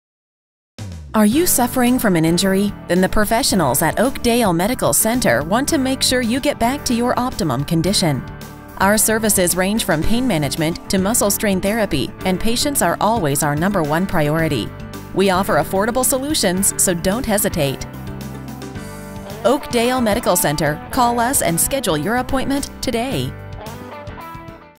Oakdale Medical Centre
Are you suffering from an injury?  Then the professionals at Oakdale Medical Centre want to make sure you get back to your optimum condition.  Our services range from pain management to muscle strain therapy, and patients are always our number one priority.  We offer affordable solutions, so don't hesitate.

2065 Finch Ave. W, Ste. 106
Toronto, ON, CA M3N 2V7